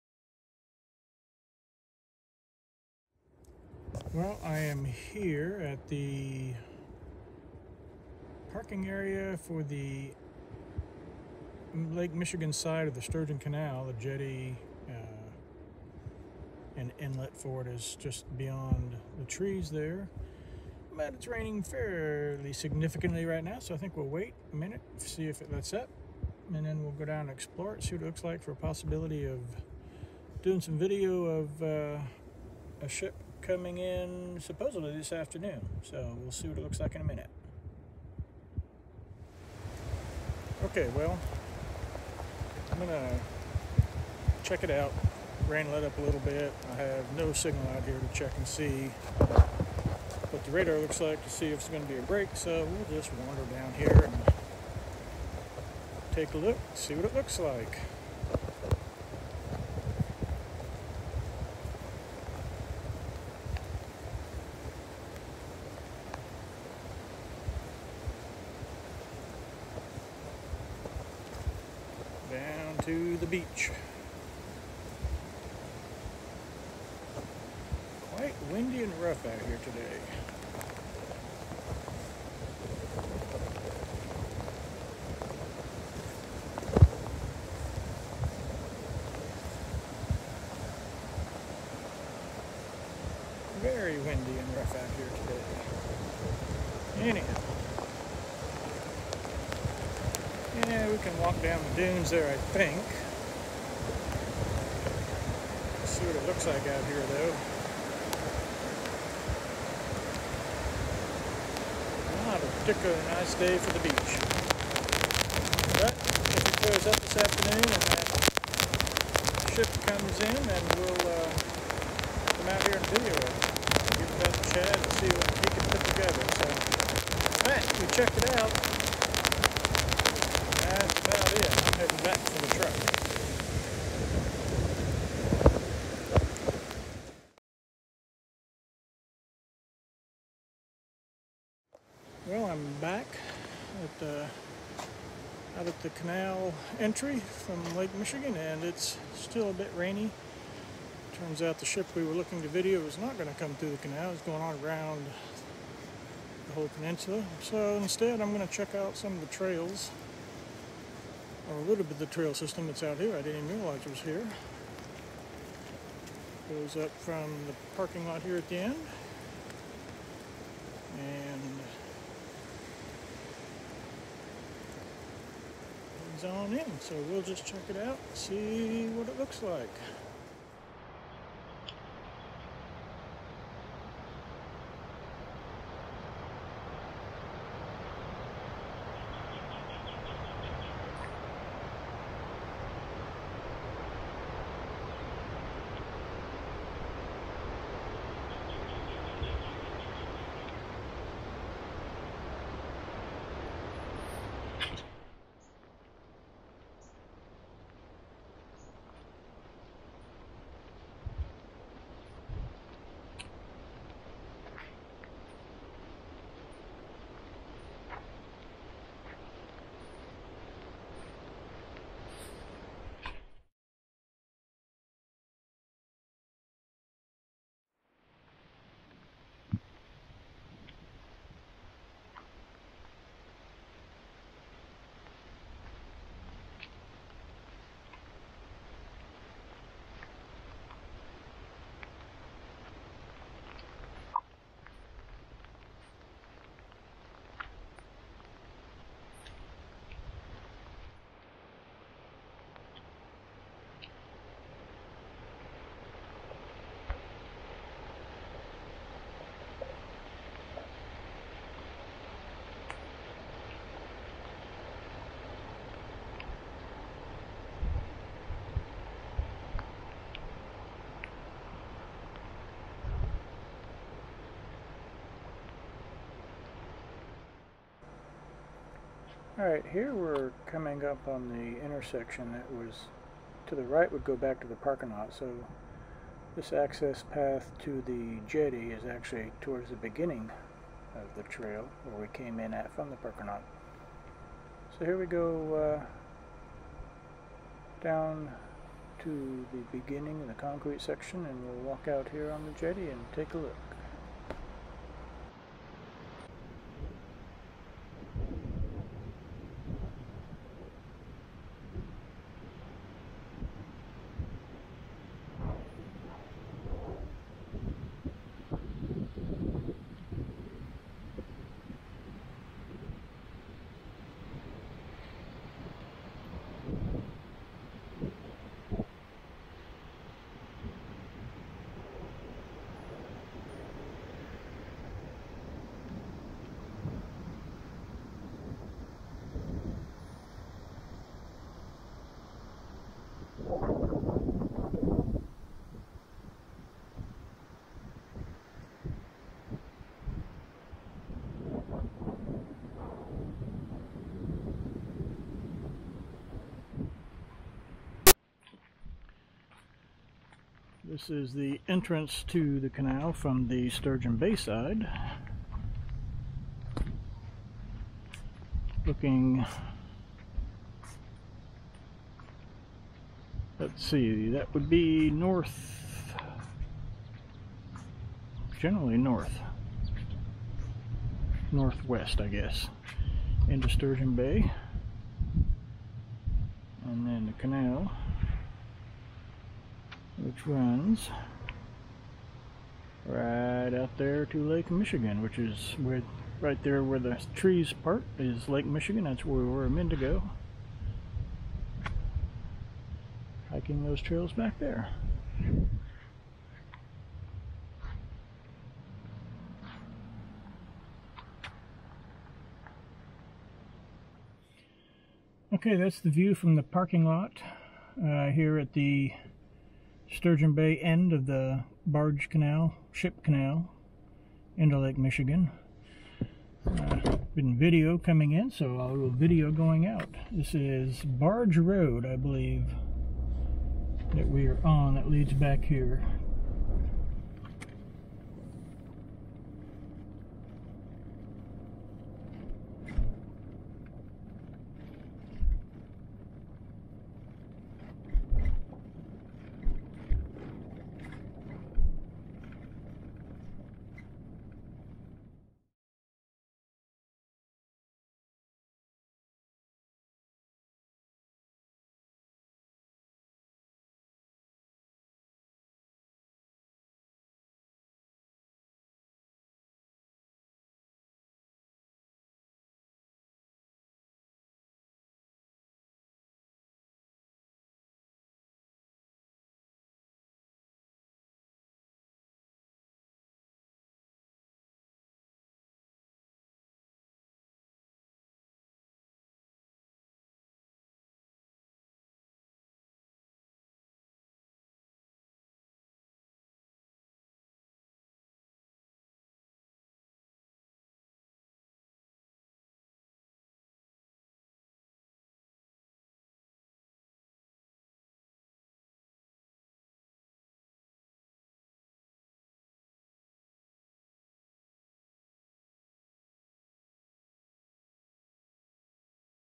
Sturgeon Bay Ship Canal, Wisconsin
A look at the Sturgeon Bay Ship Canal and some of the trails on the south side.